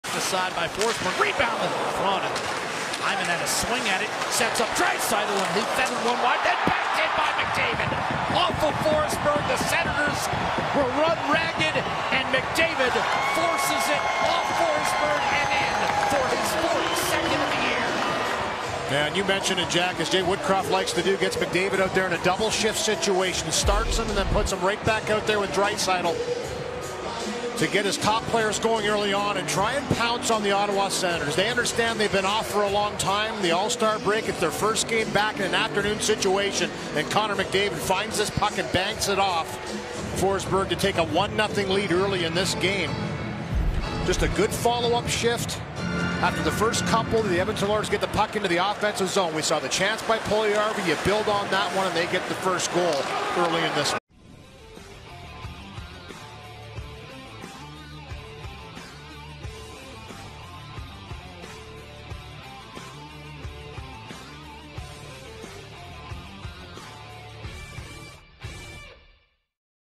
Connor McDavid Banks Home a Shifty Goal to Break the Ice
Watch as Connor McDavid and Leon Draisaitl play some ping-pong off the boards and Anton Forsberg to notch McDavid's league leading 42nd goal of the season